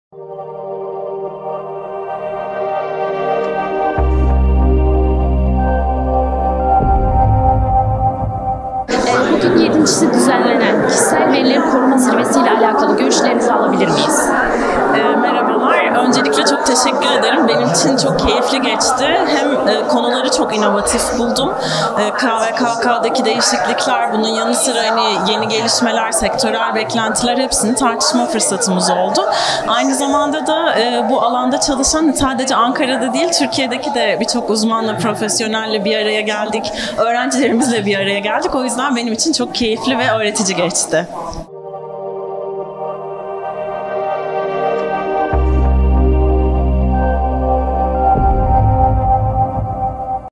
e-Safe, Sektör Paydaşlarını Bir Araya Getirdi
7’nci e-Safe Kişisel Verileri Koruma Zirvesi, “Türkiye’de ve Dünyada Kişisel Verilerin Korunmasında Yeni Gelişmeler” temasıyla 22 Mayıs 2024 tarihinde, Ankara'da, Kişisel Verileri Koruma Kurumu (KVKK) Konferans Salonu’nda düzenlendi.

Bilkent Üniversitesi Hukuk Fakültesi Öğretim Üyesi Pınar Çağlayan Aksoy, etkinlik hakkındaki düşüncelerini şu şekilde dile getirdi:

“Benim için çok keyifli geçti. Konuları çok inovatif buldum. KVKK’daki (Kişisel Verileri Koruma Kanunu) değişikliklerin yanı sıra yeni gelişmeler, sektörel beklentiler bunların hepsini tartışma fırsatımız oldu. Aynı zamanda da bu alanda çalışan sadece Ankara’da değil, Türkiye’de de birçok uzmanla, profesyonelle bir araya geldik. O yüzden benim için çok öğretici ve keyifli geçti.”

VİDEO: 790
BAŞLIK: e-Safe, Sektör Paydaşlarını Bir Araya Getirdi
RÖPORTAJ: Pınar Çağlayan Aksoy – Bilkent Üniversitesi Hukuk Fakültesi Öğretim Üyesi
ETKİNLİK: 7.e-Safe Kişisel Verileri Koruma Zirvesi
TARİH: 22 Mayıs 2024, Çarşamba
YER: Kişisel Verileri Koruma Kurumu - Ankara